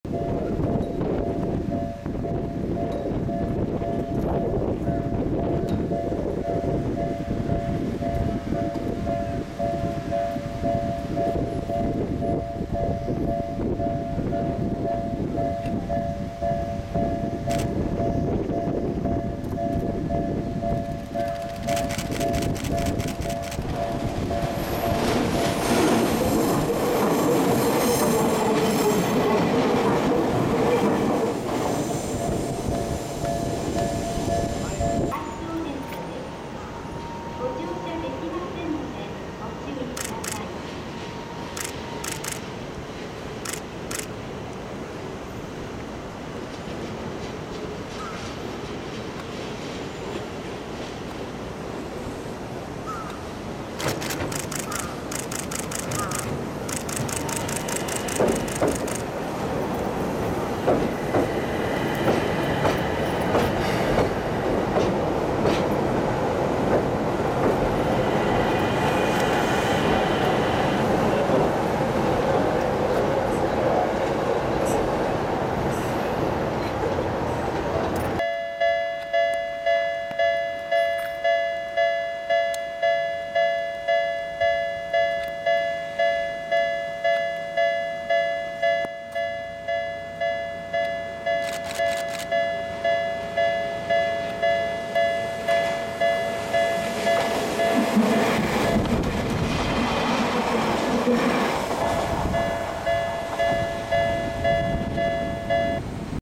東急7500系『TOQ i』 池上線検測 （デヤ7500+サヤ7590＋デヤ7550）
2012/09/06 東急池上線蒲田～蓮沼/五反田駅/池上～蓮沼